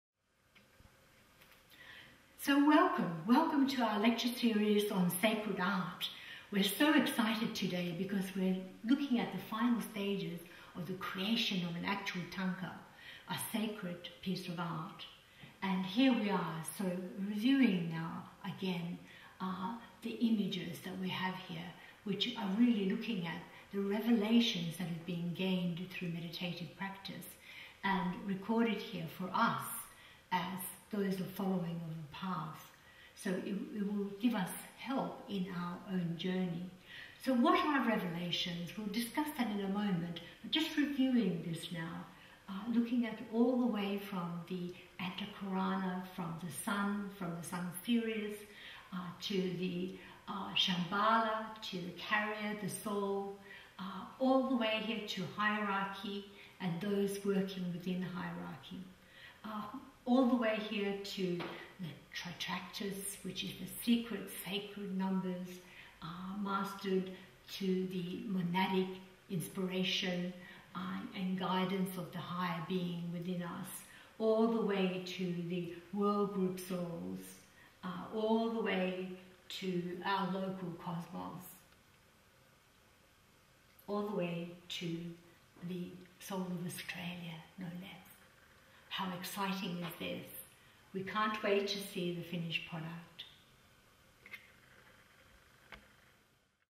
Elements of the Thanka revisited
Let us review the key elements of the Thangka, the images, symbols and revelations gained through meditative practice to represent the journey for the individual to the group, meditating on the Soul of Australia and the world group of souls, and receiving inspiration from Shamballa and the Sun Sirius.  If you would like to learn more, go